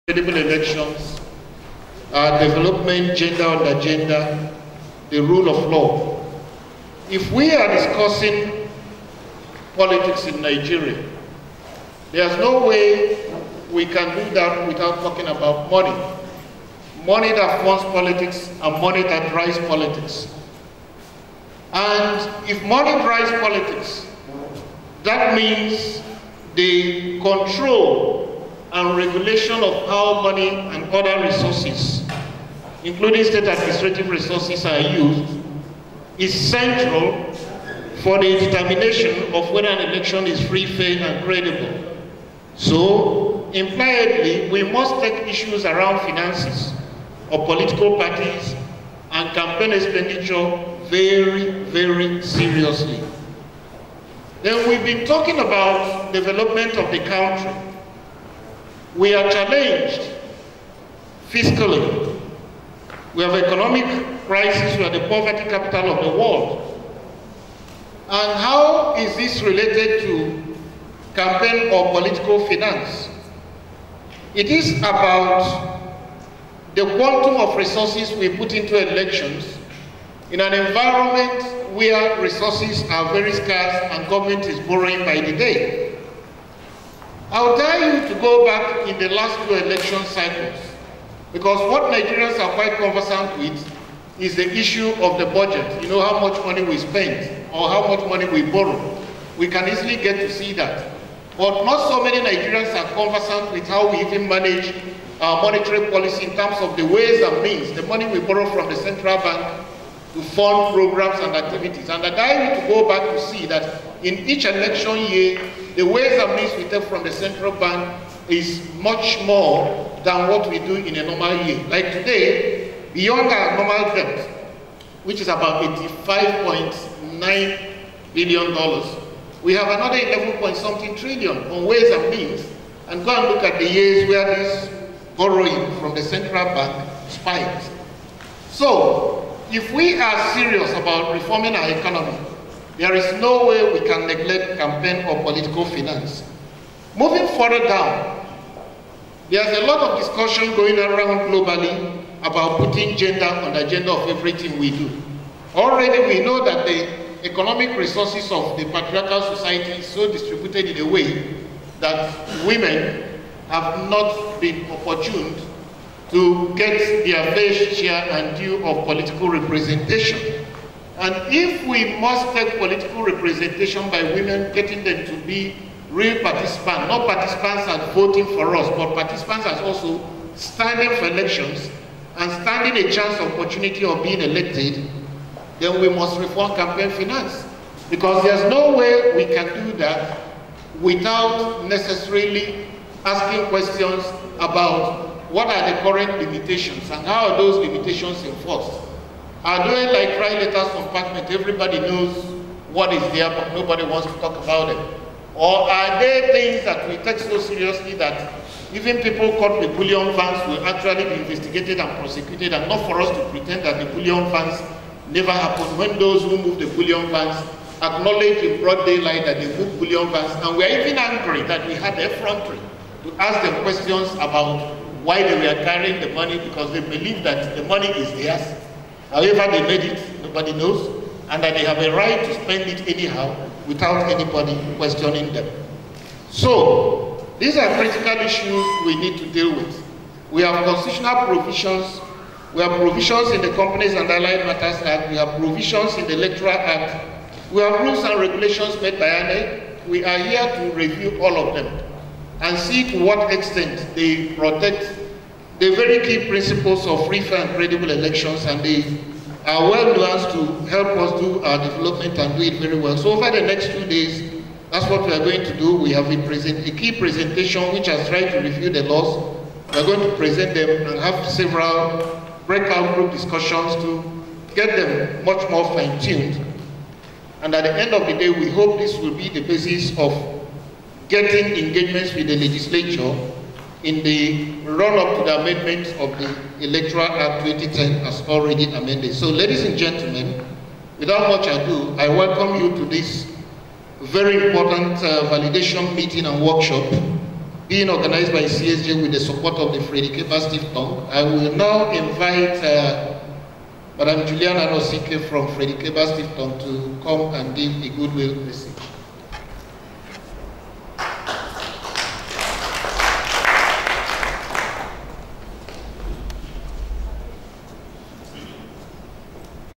Opening Remarks by Barr Eze Onyekpere
At the Validation Workshop on Reform of Political Finance Laws and Policies, organised by CSJ with the support of Friedrich Ebert Stiftung on the 15th October 2020 in Abuja.